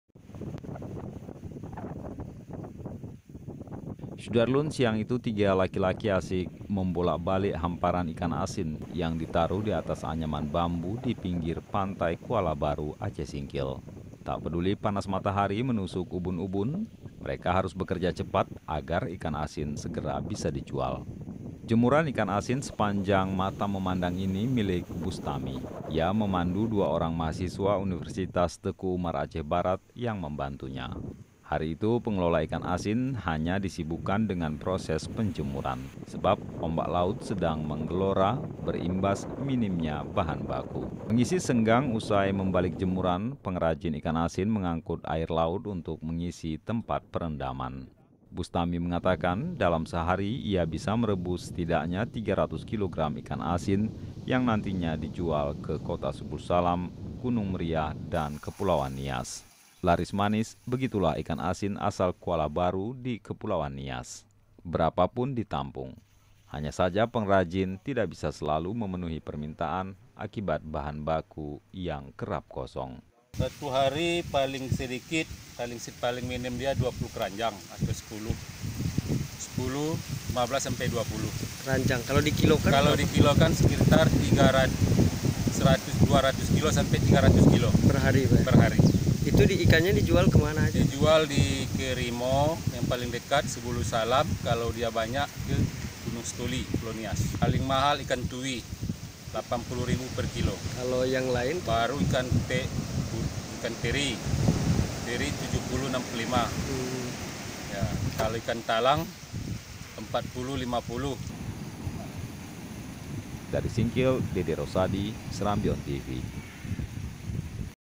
Ikan Asin Kuala Baru Aceh Singkil Laris Manis di Nias
SERAMBITV.COM, SINGKIL - Siang itu tiga laki-laki asik membolak balik hamparan ikan asin yang di taruh di atas anyaman bambu di pinggir pantai Kuala Baru, Aceh Singkil.

Tak peduli panas matahari menusuk ubun-ubun, mereka harus bekerja cepat agar ikan asin segera bisa dijual.

Jemuran ikan asin sepanjang mata memandang ini, milik Bustami. Ia memandu dua orang mahasiswa Universitas Teuku Umar Aceh Barat, yang membantunya.

Hari itu pengelola ikan asin hanya disibukan dengan proses penjemuran. Sebab ombak laut sedang menggelora berimbas minimnya bahan baku.

Mengisi senggang usai membalik jemuran, pengrajin ikan asin mengangkut air laut untuk mengisi tempat perendaman.

Bustami mengatakan, dalam sehari ia merebus setidaknya 300 kilogram ikan asin yang nantinya dijual ke Kota Subulussalam, Gunung Meriah dan Kepuluan Nias.

Laris manis begitulah ikan asin asal Kuala Baru, di Kepulauan Nias. Berapapun ditampung. Hanya saja perajin tidak bisa selalu memenuhi permintaan akibat bahan baku kerap kosong.